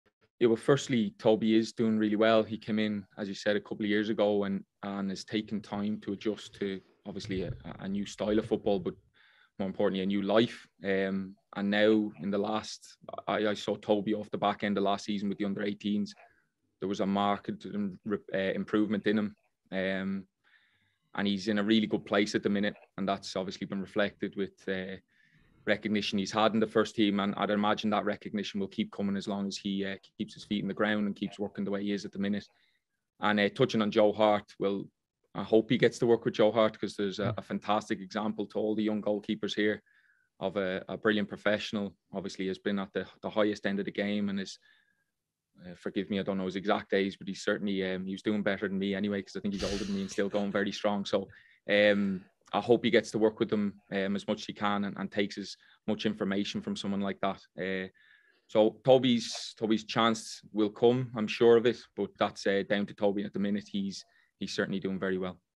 "I hope he (Tobi Oluwayemi) gets to work with Joe Hart." Darren O'Dea with A Celtic State of Mind
The birthplace of Celtic needs our help.